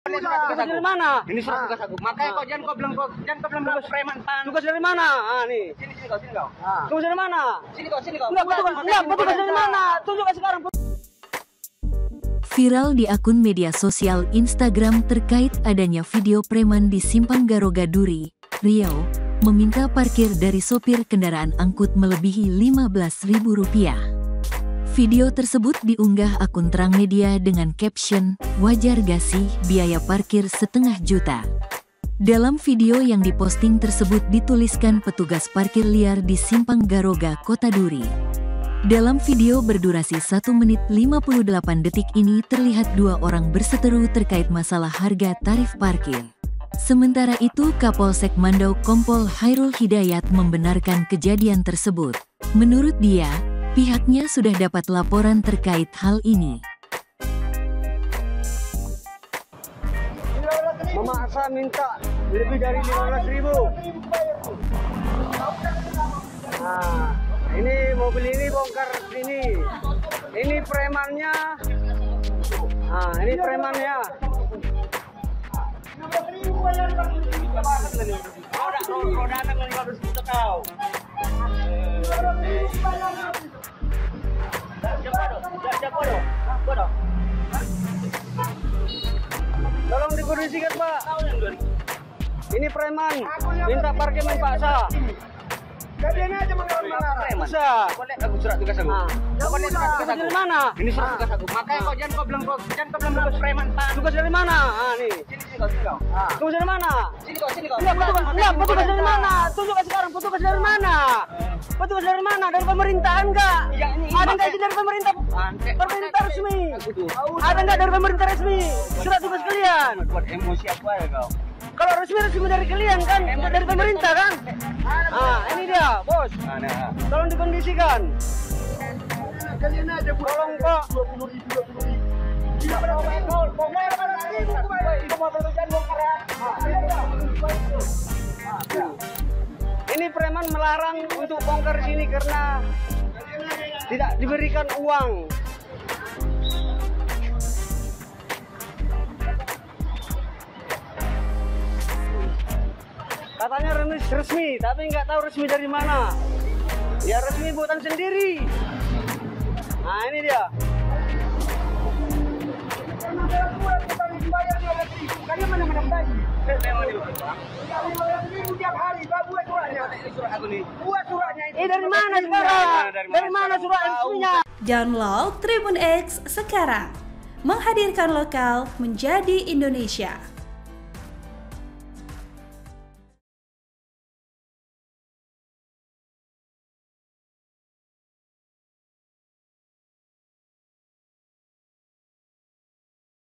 Preman di Simpang Garoga Duri Marah-marah Minta Parkir ke Supir Truk
Video tersebut diunggah akun terangmedia dengan caption 'wajar ga sih biaya parkir setengah juta.'

Dalam video yang diposting tersebut dituliskan petugas parkir liar di simpang garoga kota Duri.

Dalam video berdurasi satu menit lima puluh delapan detik ini terlihat dua orang berseteru terkait masalah harga tarif parkir.

Seorang pria dalam video tersebut terlihat menggunakan baju kaos lengan pendek dan celana pendek beradu mulut dengan seorang pemuda menggunakan baju kaos hitam celana jeans panjang dan membawa helm.

Percakapan yang terjadi pria bercelana pendek mengatakan biaya parkir yang biasa dibayarnya saat parkir di tempat tersebut hanya sebesar lima belas ribu.

Sementara pria yang menggunakan celana panjang mengatakan tarif parkir yang lima belas ribu untuk roda enam.

Sementara perekam video ini mengatakan bahwa preman minta parkir marah marah, dan memaksa untuk membayar lebih dari lima belas ribu.

Pria membawa helm inipun tersingung diseubut preman dan mengatakan punya surat tugas bakan mengeluarkan mengeluarkan kata kata kotor.

Lebih lanjut perekam video terus bercerita dalam videonya, bahwa kendaraan tersebut di larang membongkar muat sebelum diberikan uang sesuai permintaannya.

Diakhir video pemuda yang dituduh preman tesebut sempat melihatkan surat tugasnya.

Namun surat tugas yang ditunjukkan tidak jelas. Bahkan perekam menanyakan surat tugas yang ditunjukkan dari mana apakah dari pemerintahan atau tidak.

Video tersebut hingga berita ini ditulis sudah di like sebanyak 162 pengguna akun instagram, serta dikomentari 22 akun media instagram.

Salah satu komentar dari akun ardhi.adnynaa mengatakan Hajaaarr!, komentar lain dari akun erssukarti mengatakan Laporkan dan pnjarakan sja pean tdak da empati.

Sementara itu Kapolsek Mandau Kompol Hairul Hidayat membenarkan kejadian tersebut. Menurut dia, pihaknya sudah dapat laporan terkait hal ini.

Editor Video: Andri idra
Uploader :